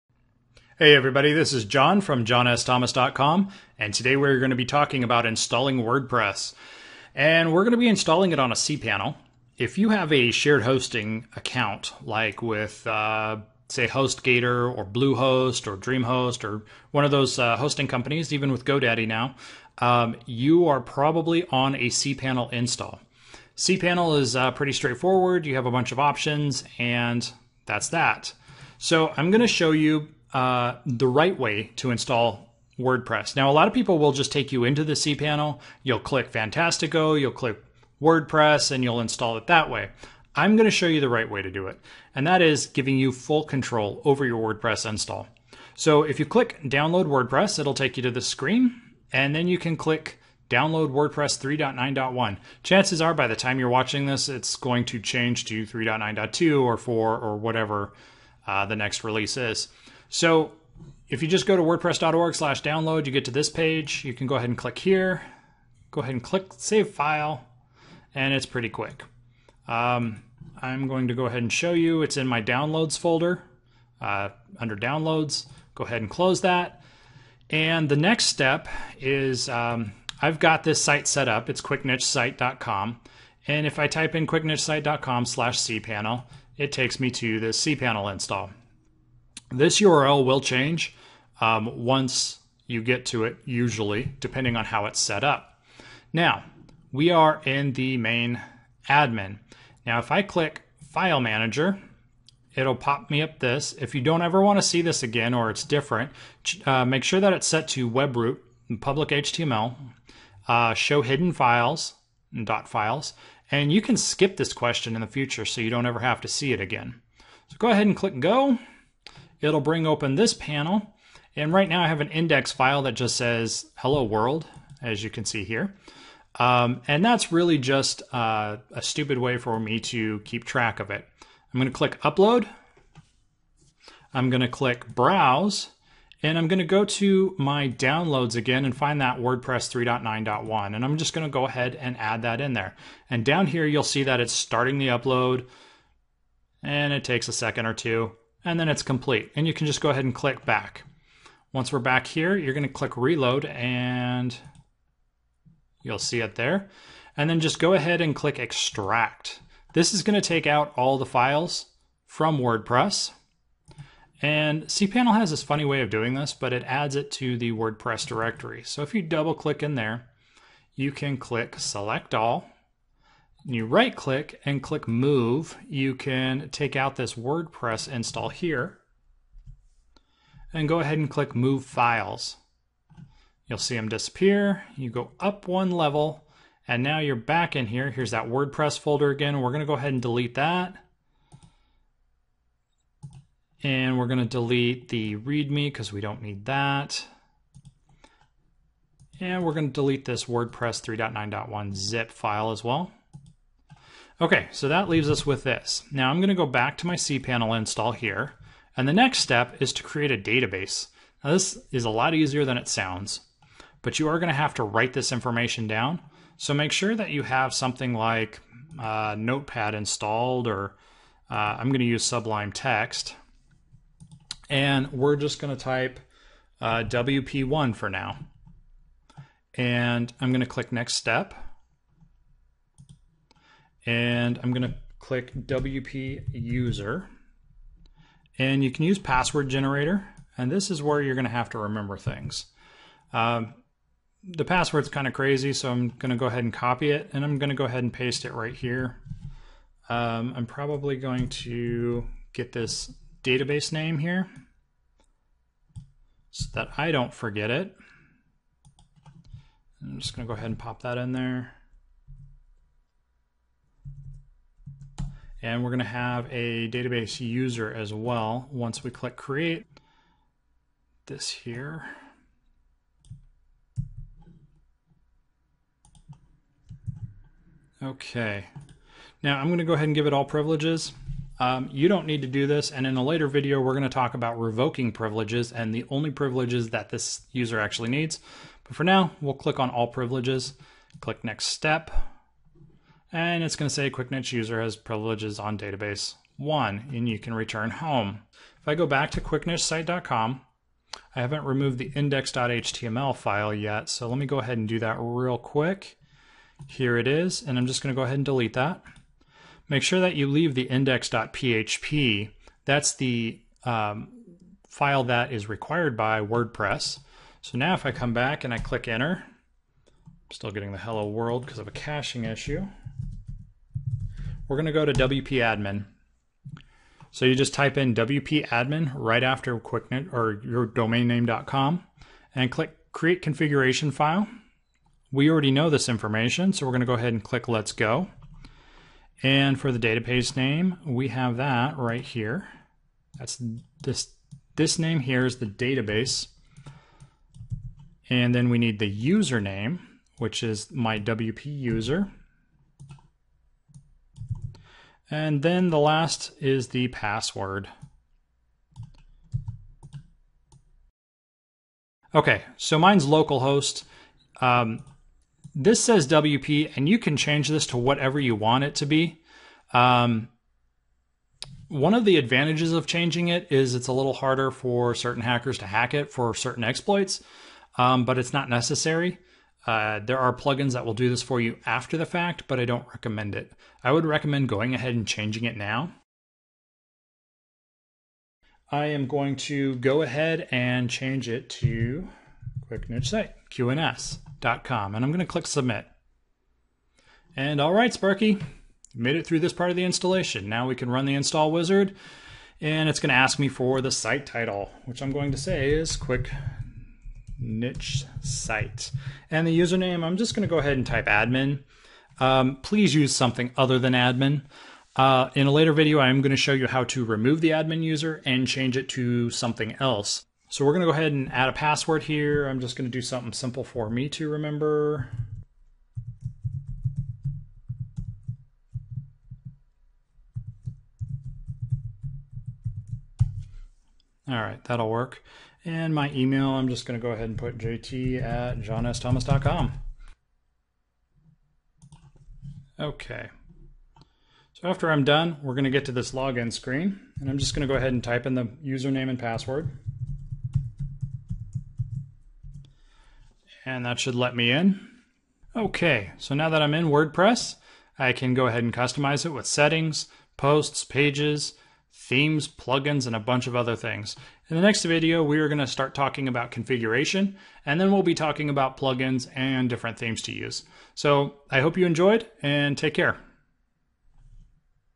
How to install WordPress on CPanel 2016
Learn how to install WordPress on CPanel the right way. Installing WordPress is fast and easy, even without using automated software.

How to install WordPress on CPanel